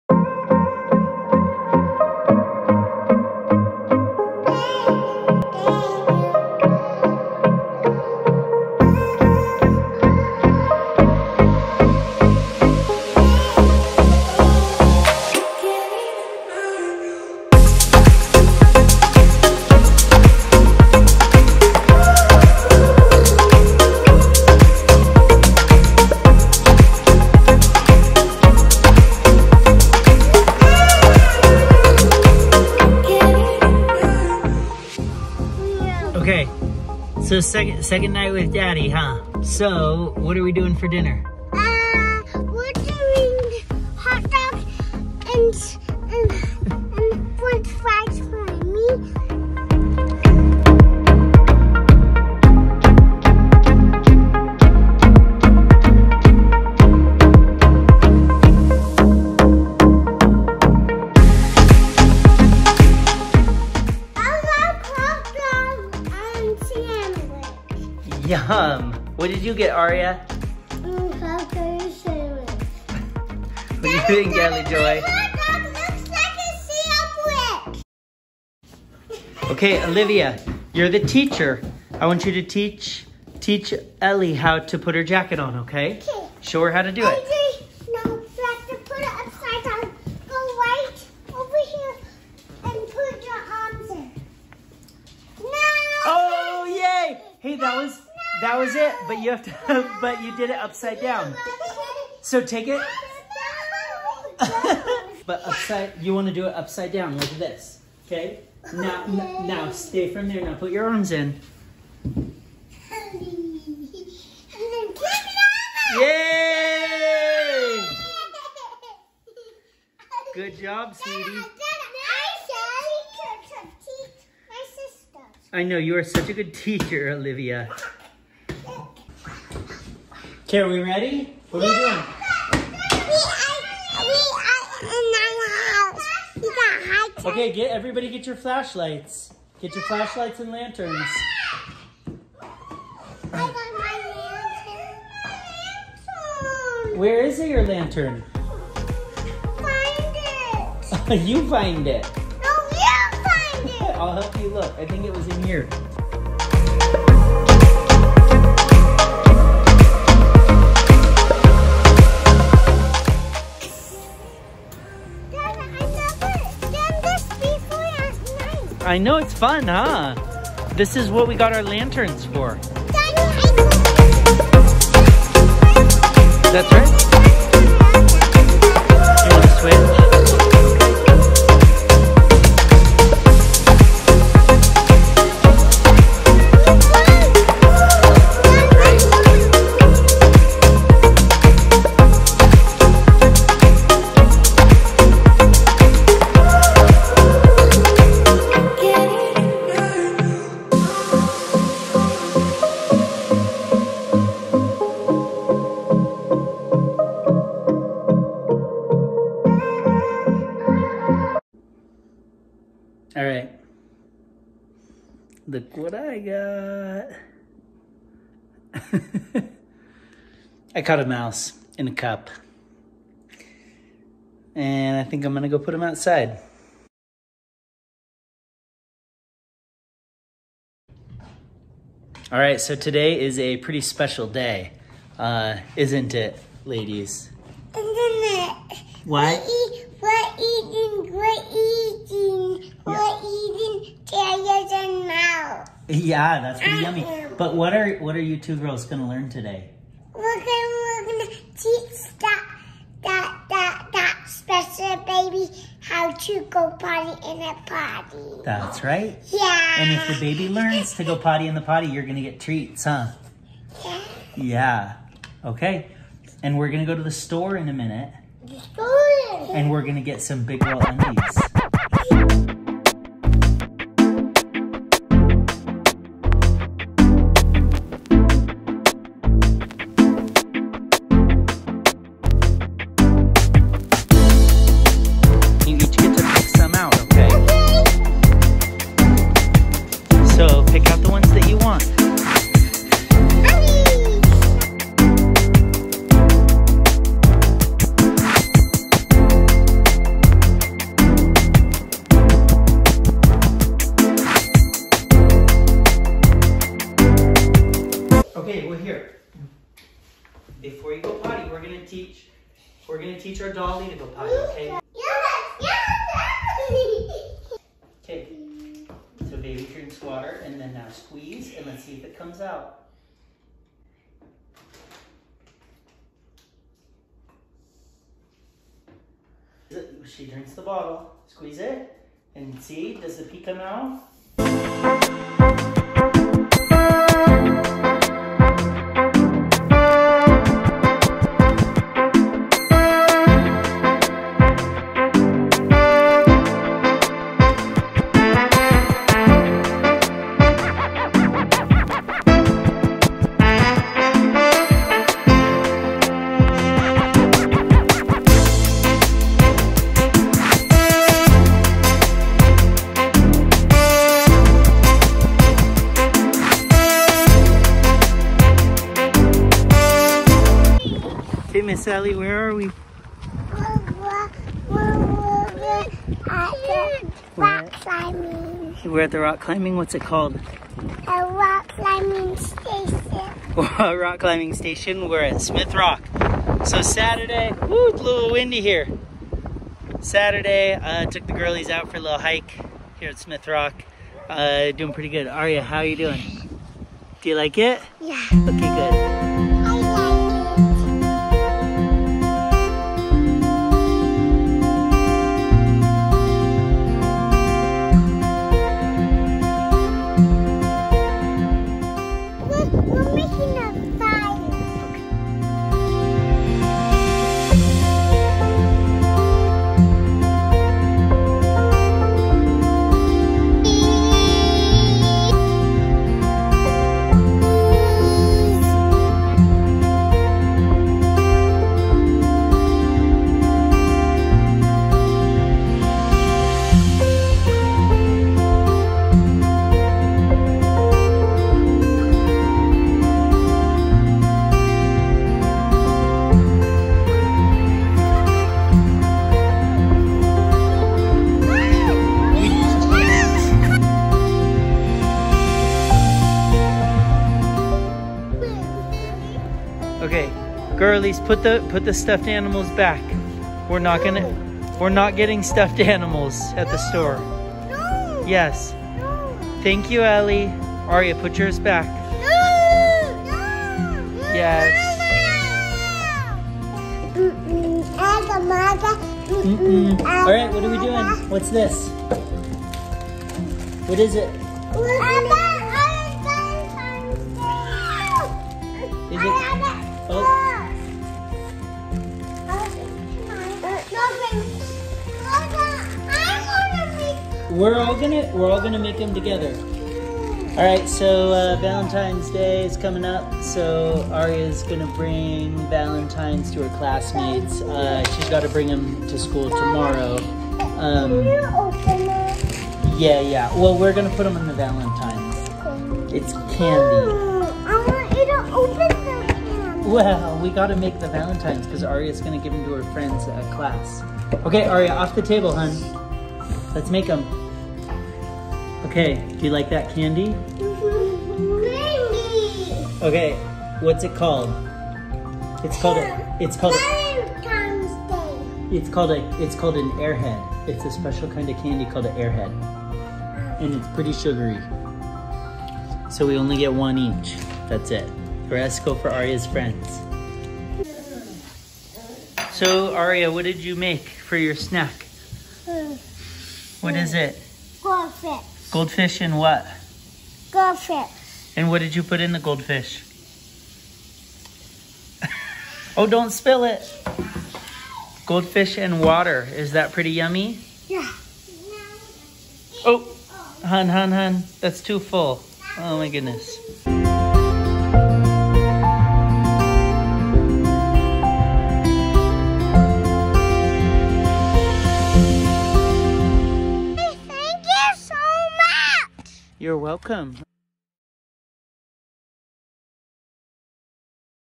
Ten Days with Dad
Ten days of night hikes, potty training, smith rock and more. Feb 3-12, 2021.